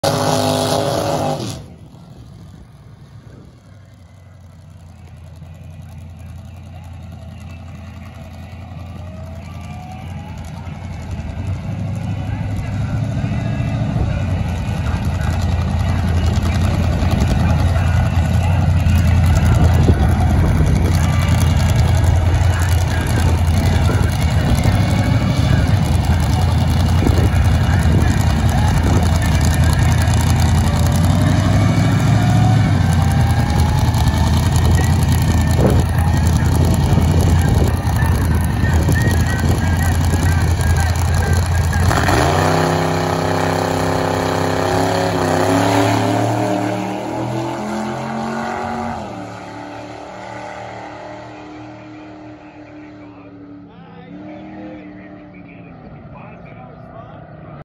Ben Elston
Ben elston at the wheel of the dragster